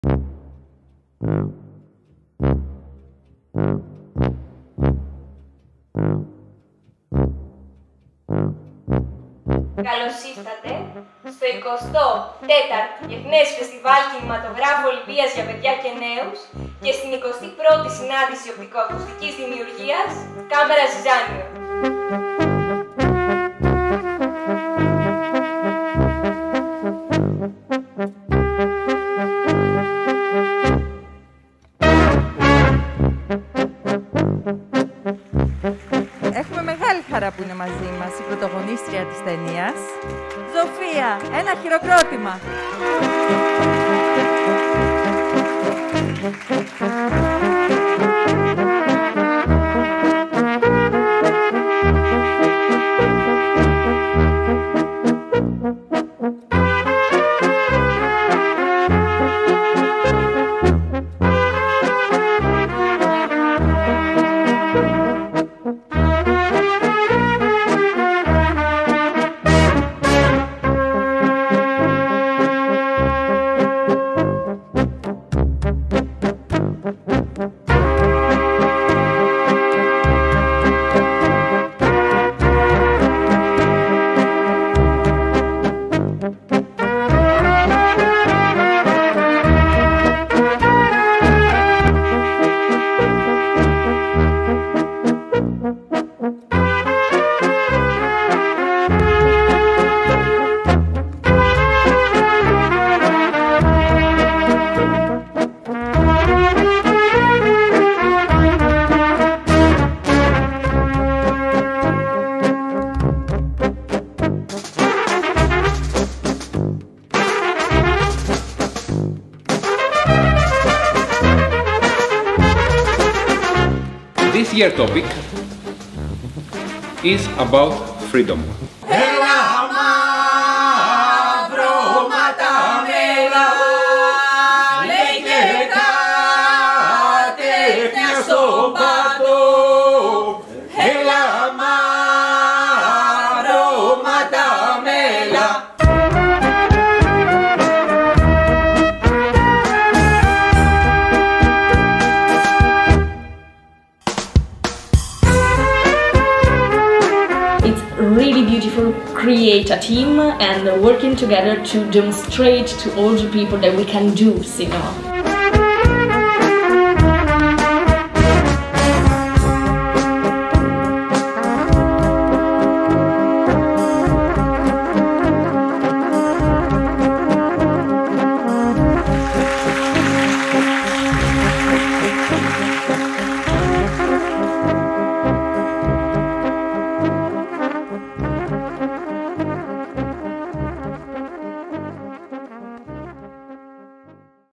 24th Olympia IFF/ 24ο Φεστιβάλ Κινηματογράφου Ολυμπίας - clip
Το Διεθνές Φεστιβάλ Κινηματογράφου Ολυμπίας για Παιδιά και Νέους και η Ευρωπαϊκή Συνάντηση Νεανικής Οπτικοακουστικής Δημιουργίας Camera Zizanio επέστρεψαν στον φυσικό τους χώρο, στις αίθουσες και τους δρόμους του Πύργου και της Αμαλιάδας το διάστημα 27/11-3/12/2021 για την 24η και την 21η τους διοργάνωση αντίστοιχα.

Olympia International Film Festival for Children and Young People and the European Meeting of Youth Audiovisual Creation Camera Zizanio returned to their home, the cinemas and streets of Pyrgos and Amaliada, Greece, for their 24th and 21st edition respectively, November 27-December 3, 2021.